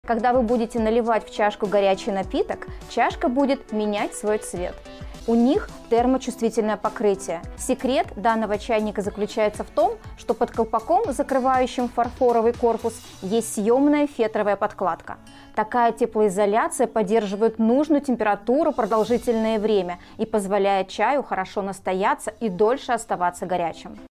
ЧАЙНЫЙ СЕРВИЗ С СЕКРЕТОМ ☝️ Guy Degrenne Salam
Это не обычный чайный сервиз, а со своими секретами! Чашки, меняющие свой цвет и утеплённый чайник вы точно не увидите каждый день. Эта фарфоровая посуда подойдёт даже для чайных церемоний.